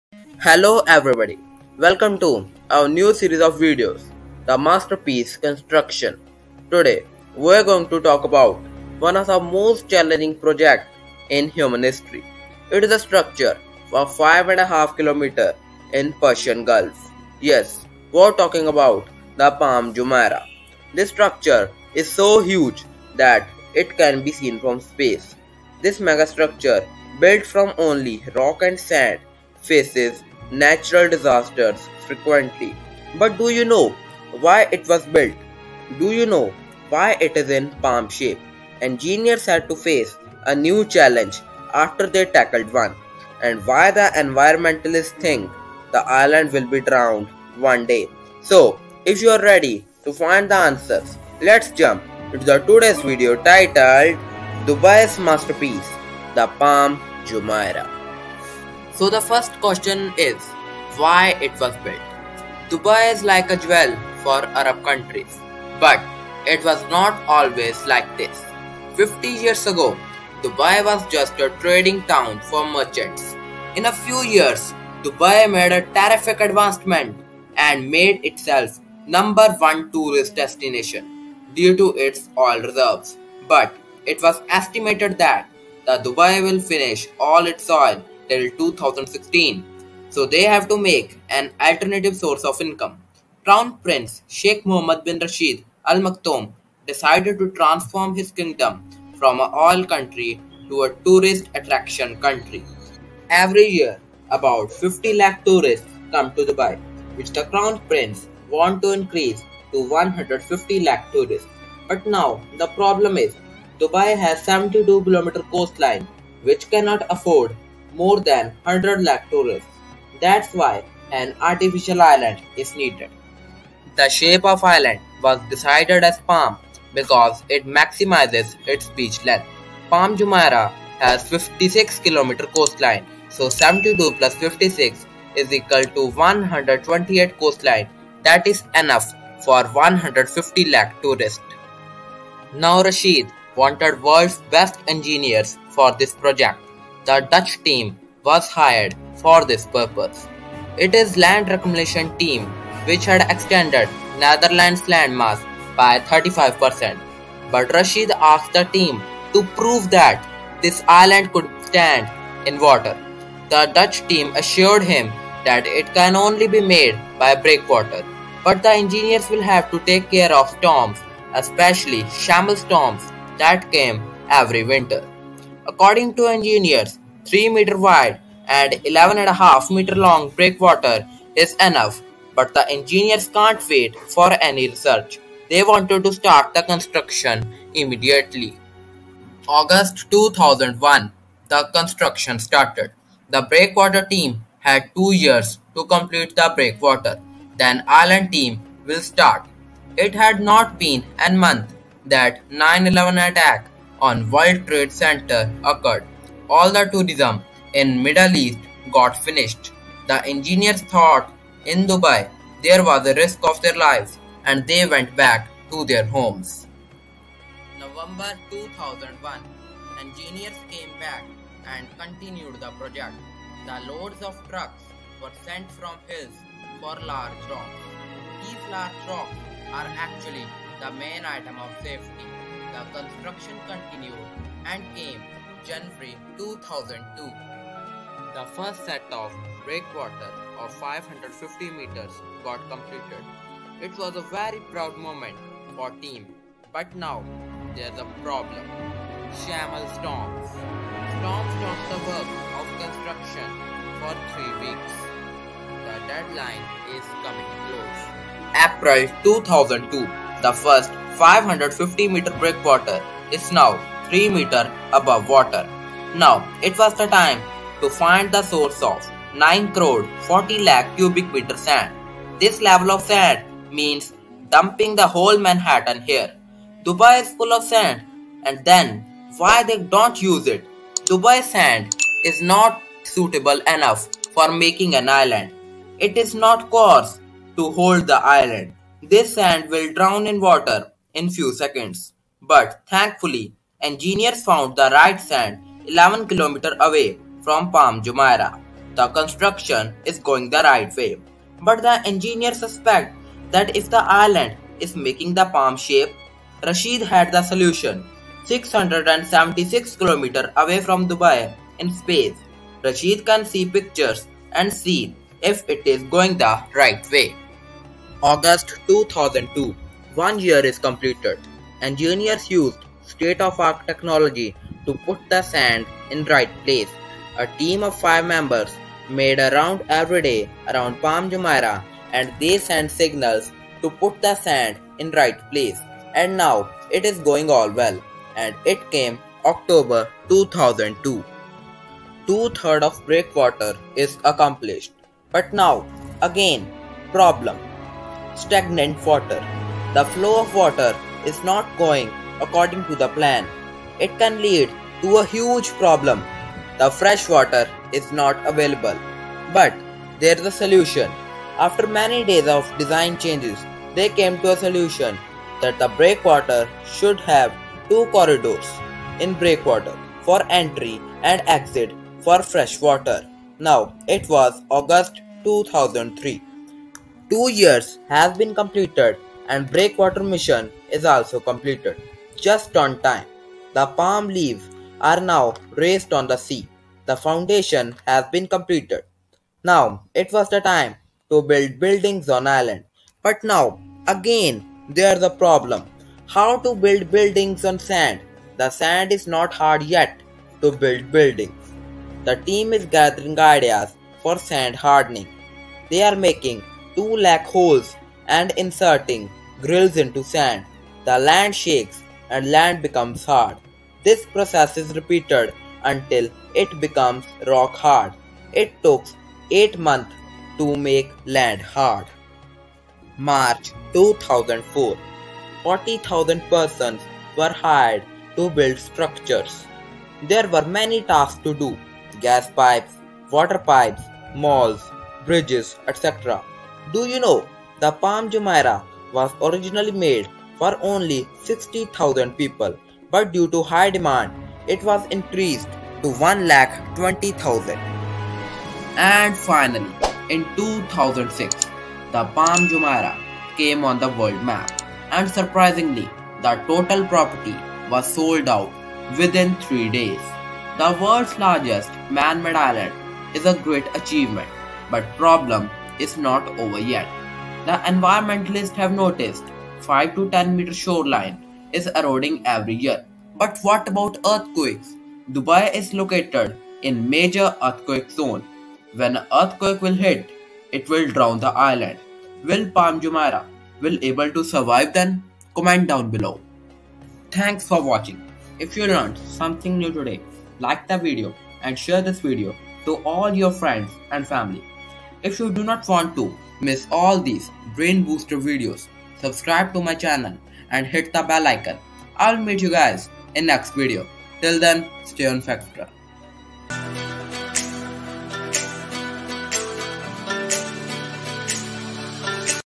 How the world's largest man-made island was constructed | Palm Jumeirah Dubai | Factara
How the world's largest man-made island was constructed?

Hello guys,
The Palm Jumeirah is a manmade archipelago in Dubai, in the United Arab Emirates, created using land reclamation which extends into the Persian Gulf. It has an estimated population of 10,500 as of 2016. It is located on the Jumeirah coastal area of the emirate of Dubai, in the United Arab Emirates

How the Palm Jumeirah was constructed ? Engineers had to face a lot of problems while building the island. Did Engineers tackle it ? How did they able to tackle it?